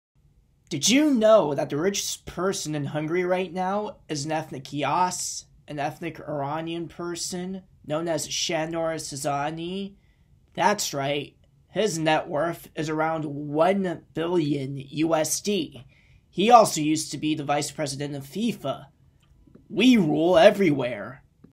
On The Richest Man In Hungary (An Iranian!)
The Richest Man In Hungary is Sandor Csanyi, an ethnic Jasz/Iranian person.